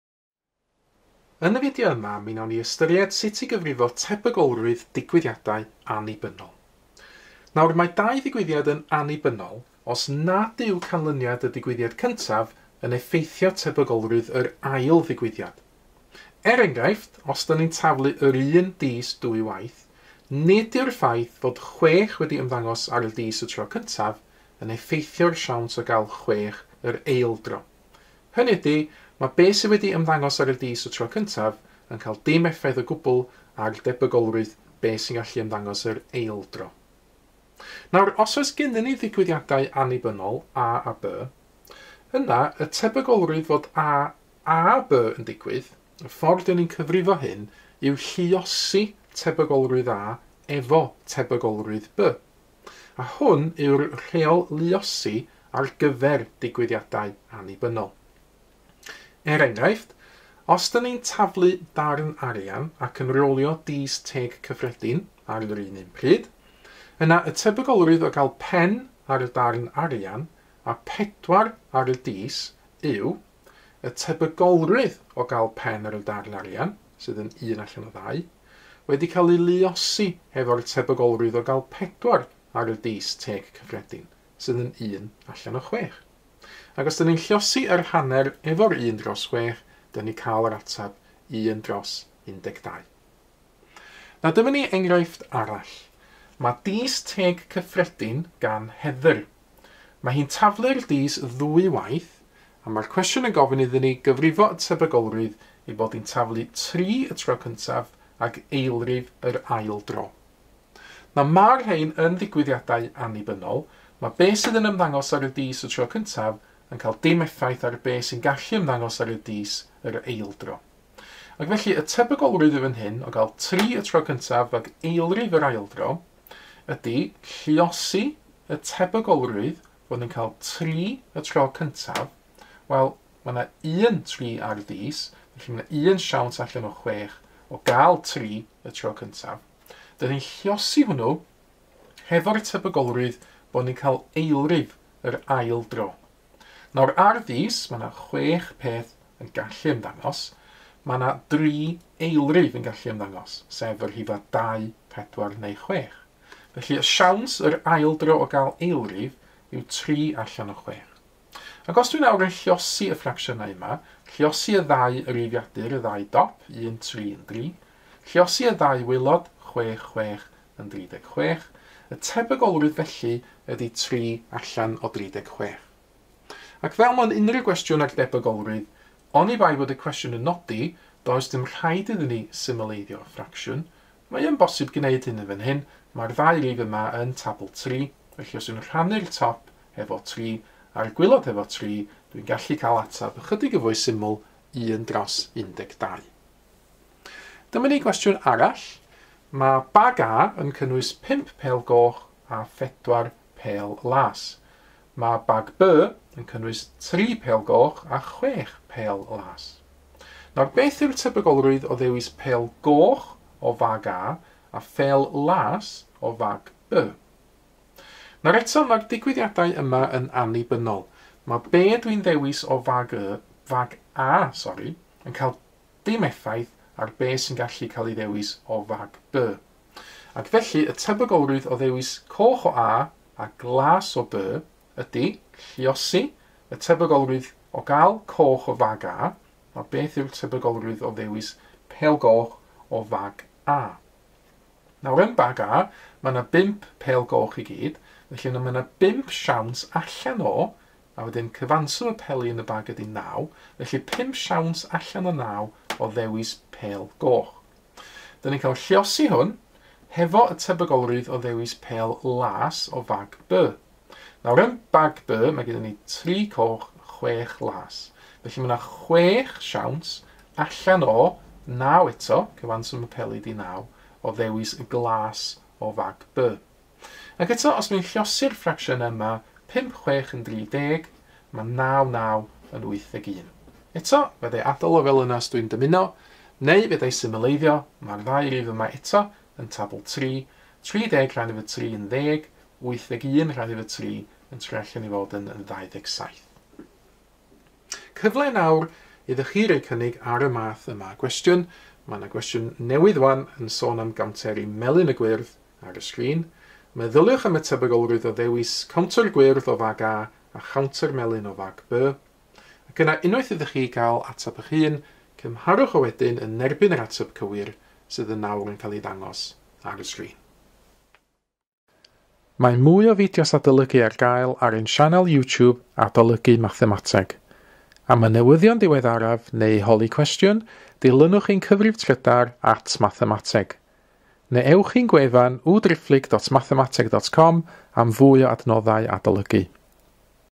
[381 M/C] (349 S/2) Digwyddiadau Annibynnol
Sut ydych yn cyfrifo tebygolrwydd digwyddiadau annibynnol?